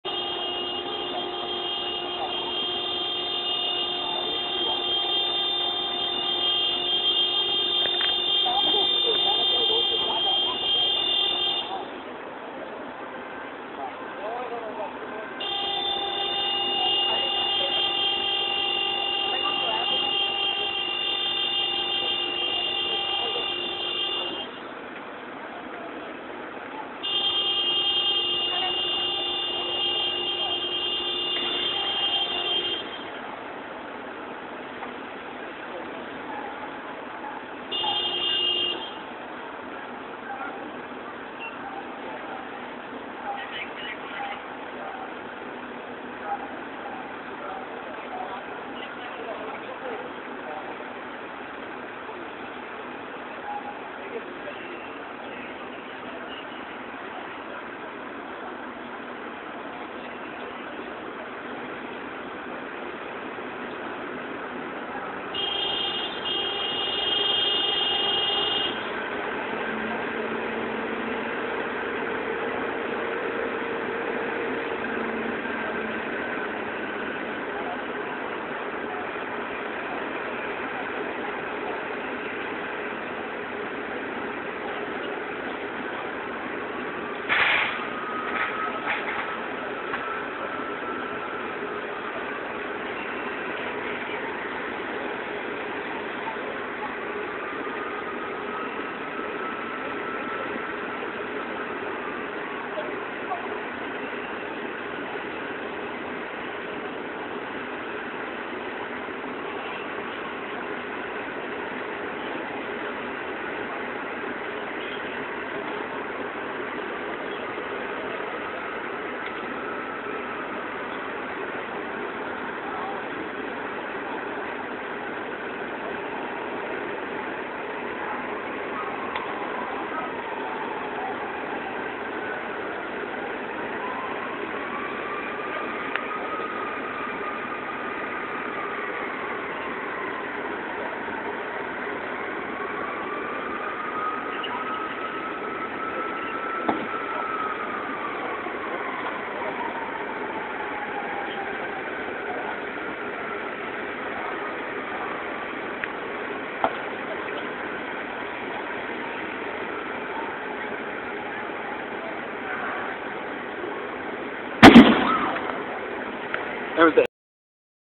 Telephone Pole falls on Car, which Bursts into Flames
In a freak accident (31 May, 2012), a transformer-bearing telephone pole snapped half-way up and fell on a driving Toyota Celica outside A&J's dim sun restaurant in Annandale Virginia -- taking out power for the entire neighborhood, and causing the car to burst into flame.  The driver escaped with only minor glass cuts on his hands.

This video shows the car being slowly engulfed in flame, while the fire brigade figures out how to cut off power to the electric lines.  The explosion at the end is just the airbag.